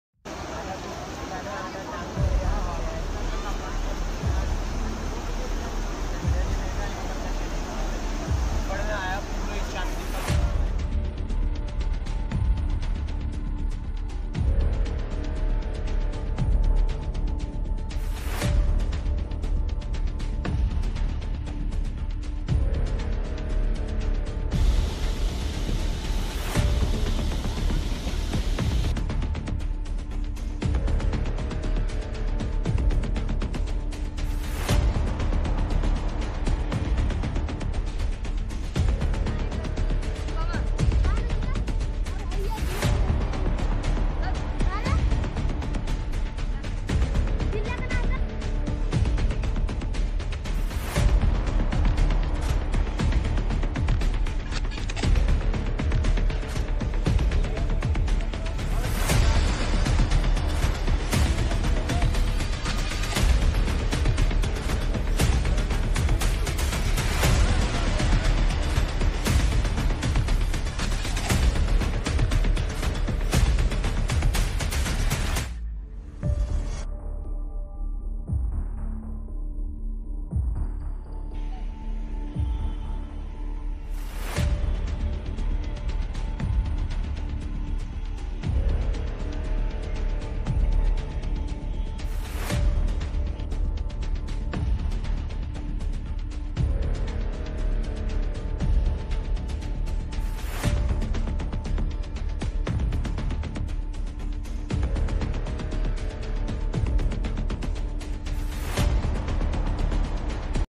After receding for a day, Ganga rises again in Varanasi, India.
Hundreds of people left their homes due to the flooding of the Ganges River!